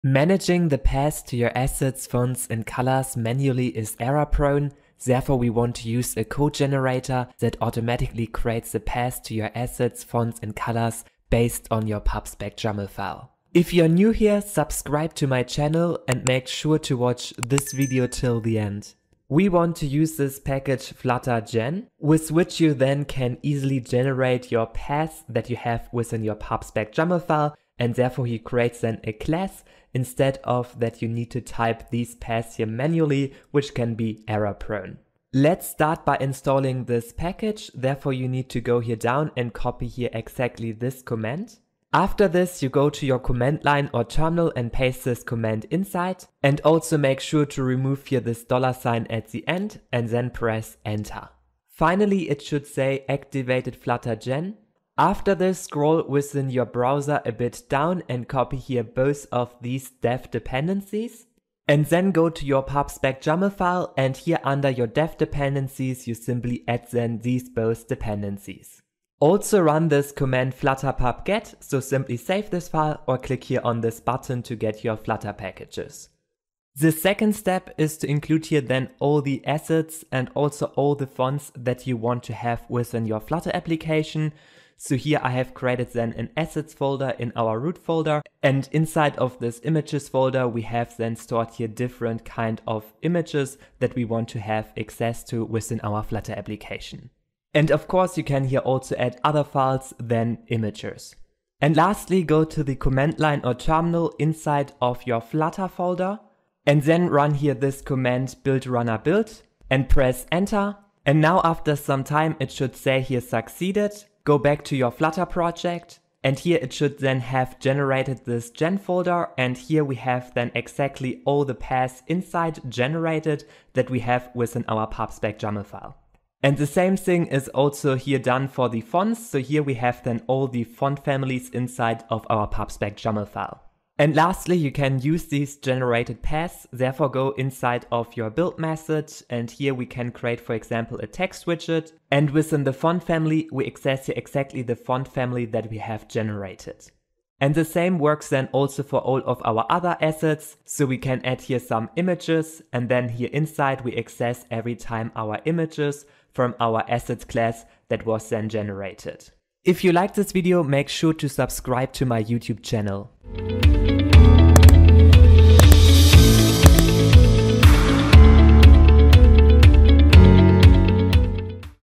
Flutter Tutorial - Generate Assets & Fonts Automatically | Flutter Gen
Use Flutter to generate assets, fonts and colors automatically from pubspec.yaml with Flutter Gen.

TIMELINE
0:00 Introduction Generate Assets & Fonts Automatically
0:35 Setup Flutter Gen Package
1:16 Generate Assets & Fonts Paths
2:14 Use Generated Assets & Font Paths